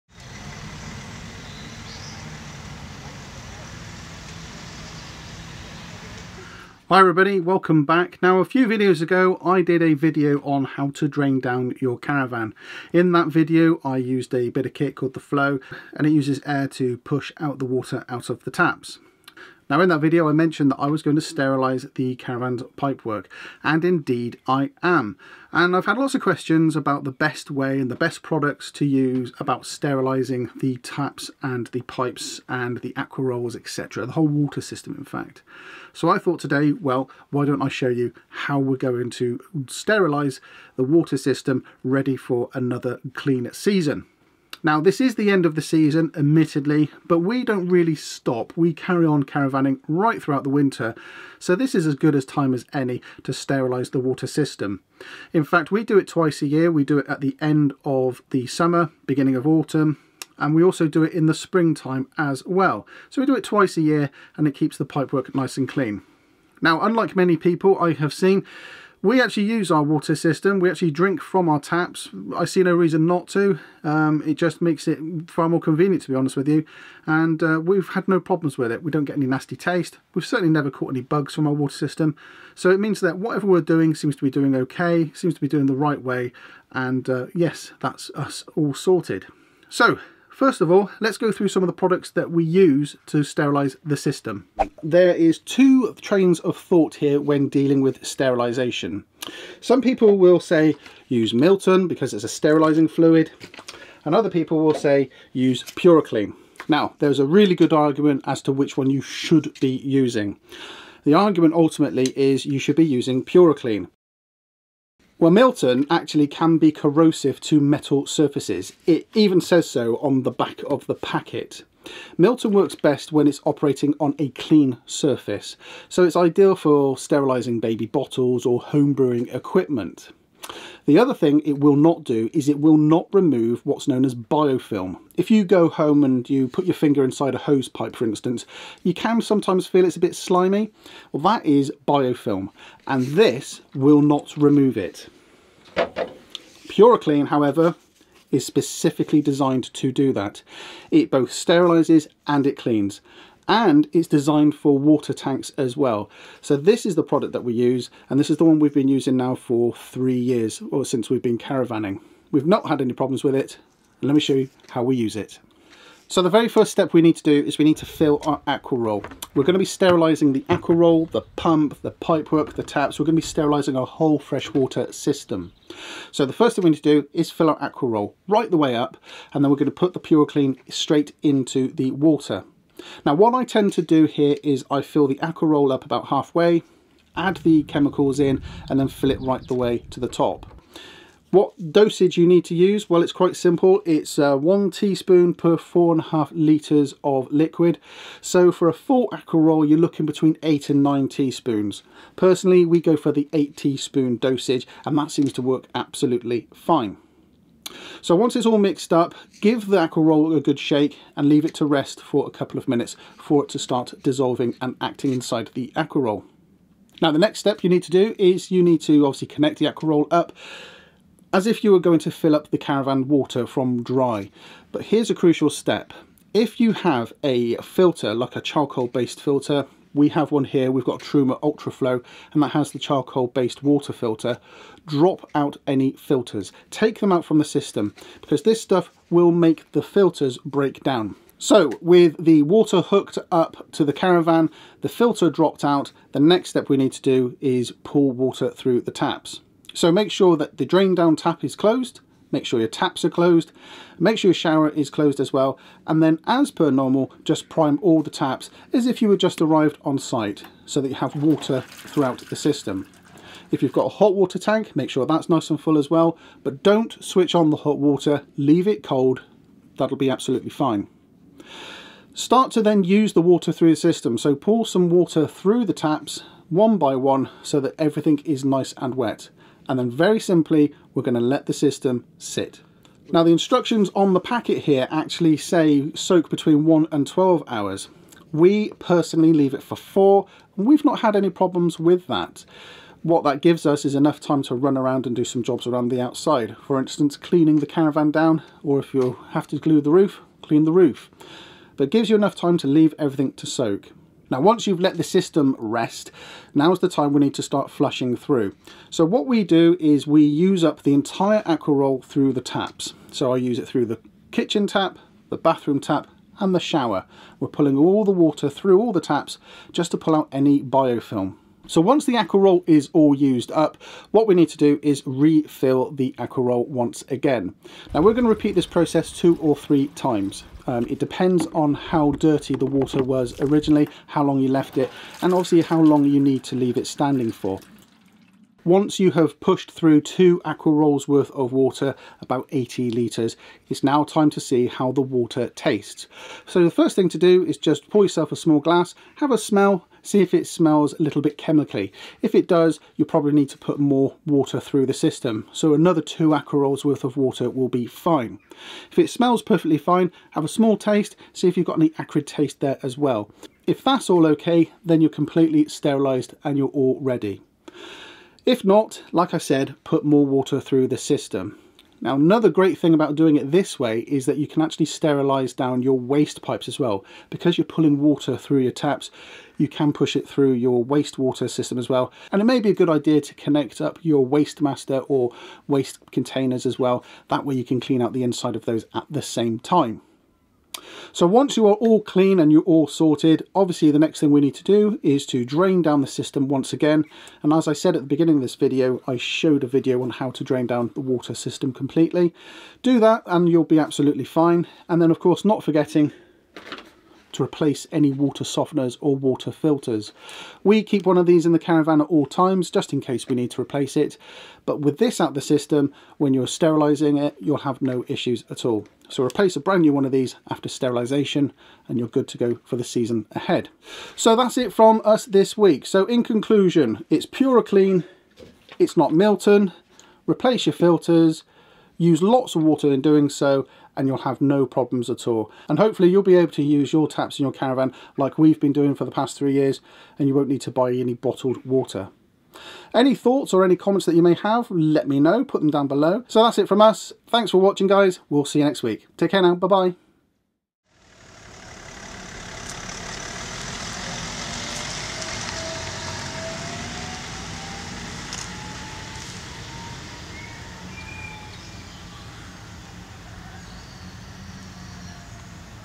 Caravan water sterilisation
A clean water supply is essential in a caravan, and here are some tips on getting it right.

It costs you no extra and it really helps us invest into our channel.

Truma Ultraflow filter